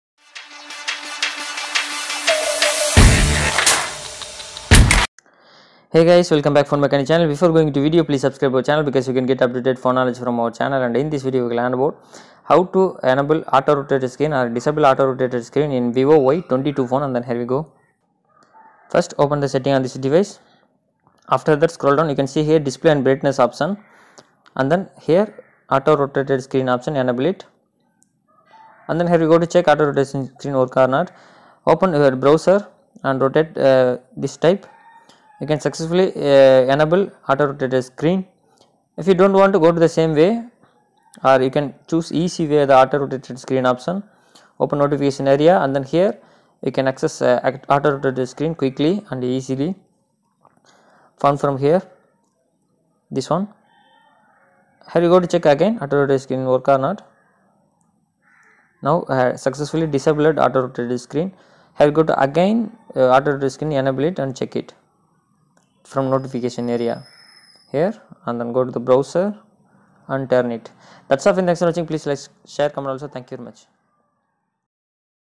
How to Enable Auto rotation screen in VIVO Y22|Auto Rotate Screen In Vivo Y22, Rotate Screen Setting
In today's video i am going to show How to Enable Auto rotation screen in VIVO Y22 phone full guide watch full video for acqurecy. unless you have to face a problem..